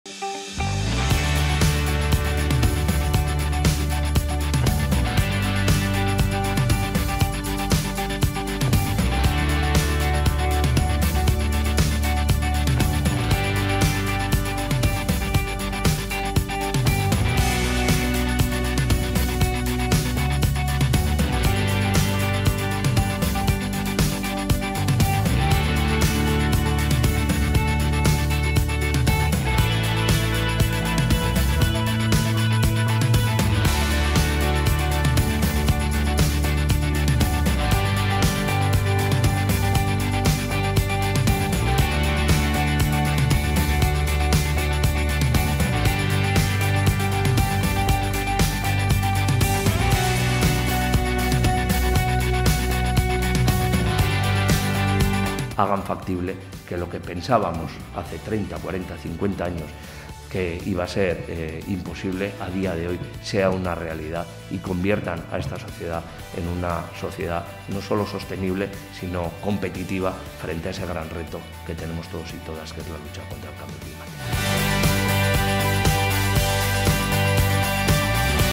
Eneko Andueza participa en el Congreso BizitzaJokoan de Mugarik Gabe
El compromiso de los gobiernos en los que estamos presentes los Socialistas Vascos es firme con las políticas públicas que hagan de nuestra sociedad un espacio sostenible y competitivo en la lucha contra el cambio climático.